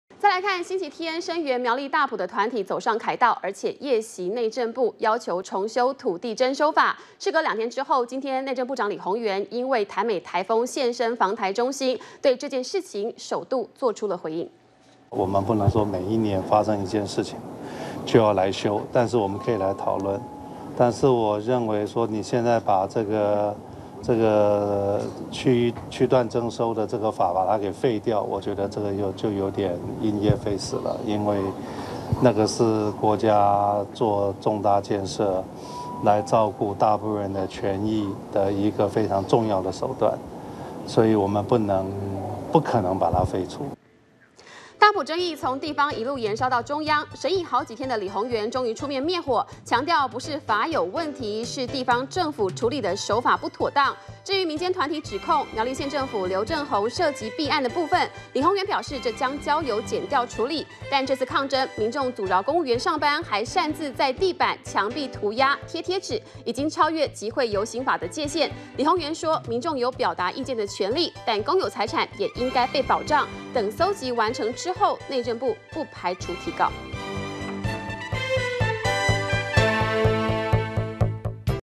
【2013.08.21】聲援大埔要修法 李鴻源:因噎廢食 -udn tv
星期天聲援苗栗大埔的團體，走上凱道，而且夜襲內政部要求重修土地徵收法，事隔兩天後，今天，內政部長李鴻源因為潭美颱風，現身防颱中心對這件事情，首度做出了回應。完整內容 , FB , Twitter , G+